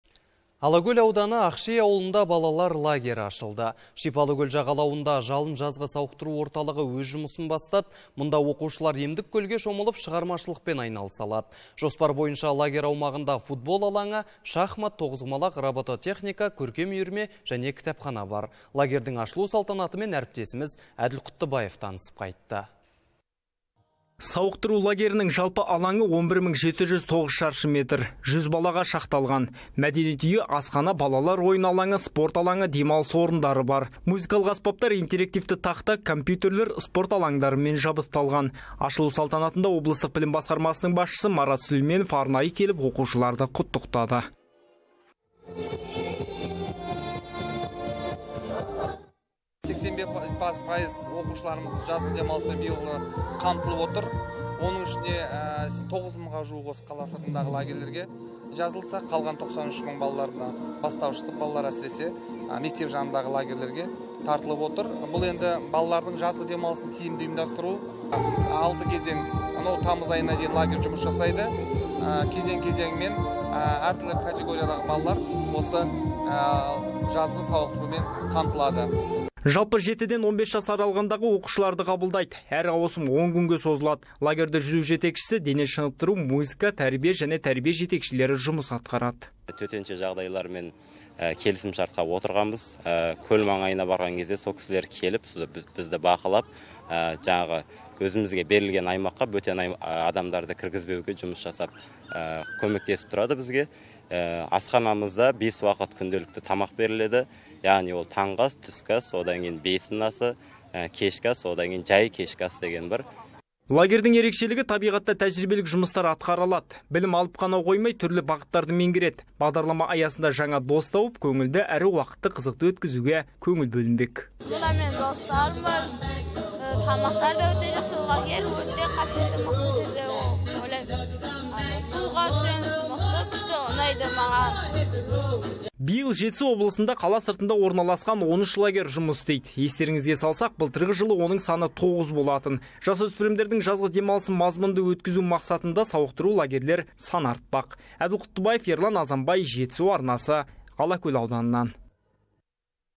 Алакөл ауданы Ақши ауылында балалар лагері ашылды
Алакөл ауданы Ақши ауылында балалар лагері ашылды. Шипалы көл жағалауында «Жалын» жазғы сауықтыру орталығы өз жұмысын бастады. Мұнда оқушылар емдік көлге шомылып, шығармашылықпен айналыса алады. Жоспар бойынша лагерь аумағында футбол алаңы, шахмат, тоғыз құмалақ, робототехника, көркем үйірме, кітапхана орнатылған.